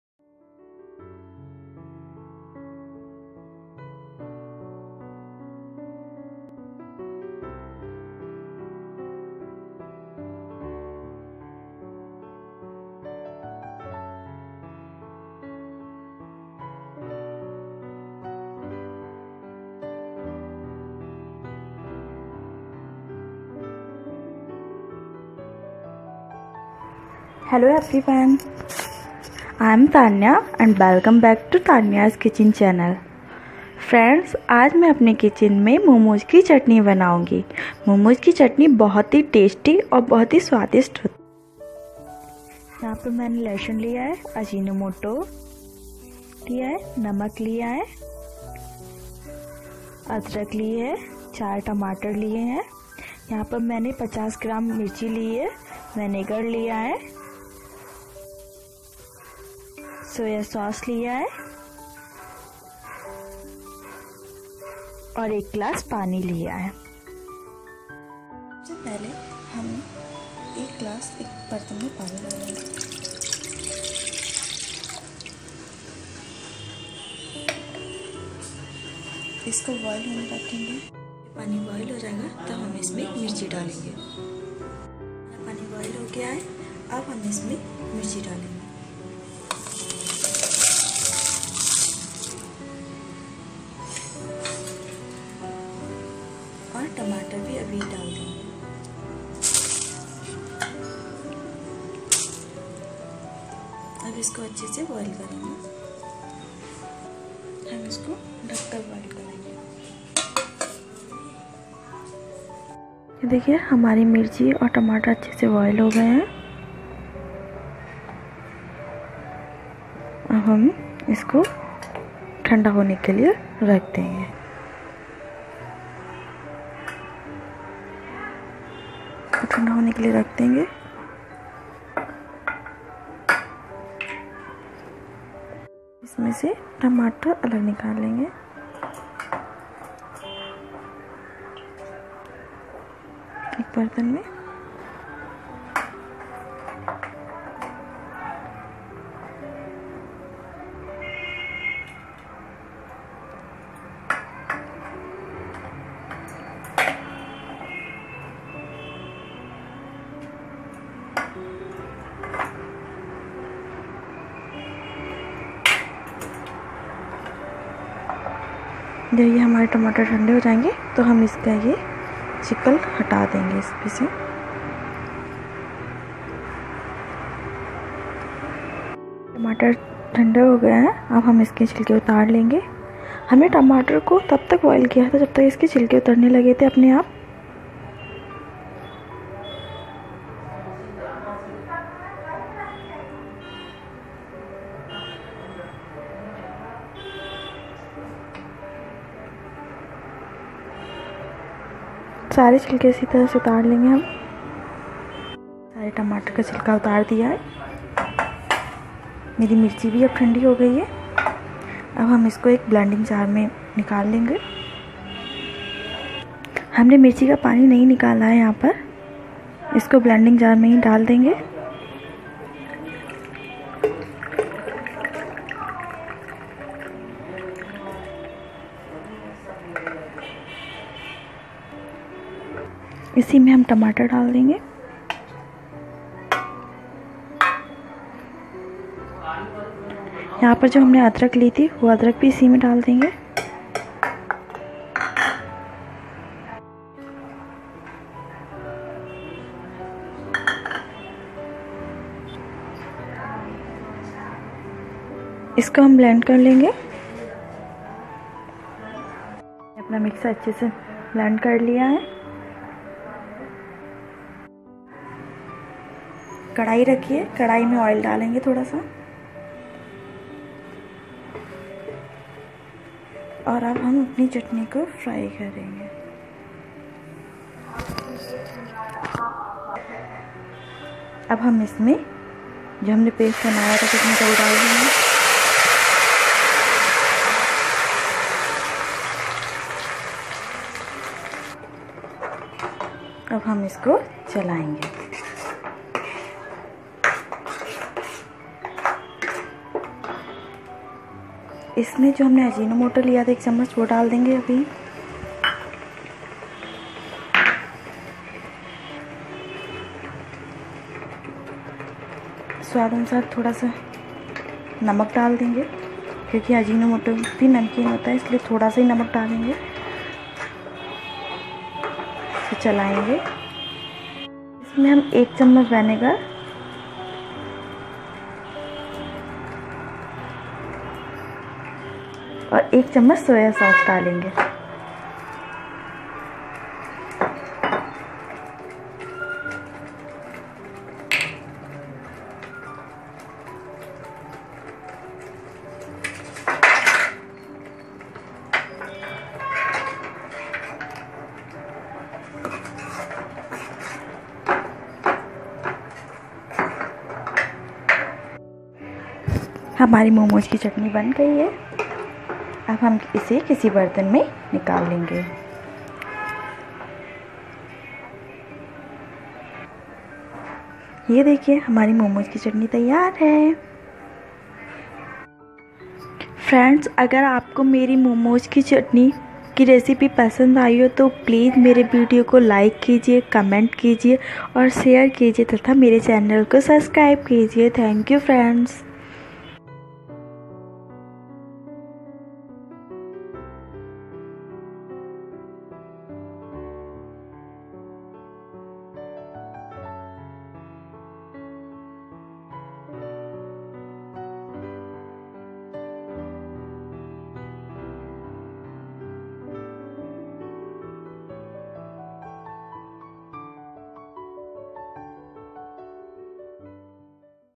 How to make Veg Momos Red Chutney in Hindi - A Quick Recipe of Momos Chutney
Do you want to make spicy veg momos red chutney then this video is for you, it is a step by step instructional video of quick recipe to make Momos Chutney at home in Hindi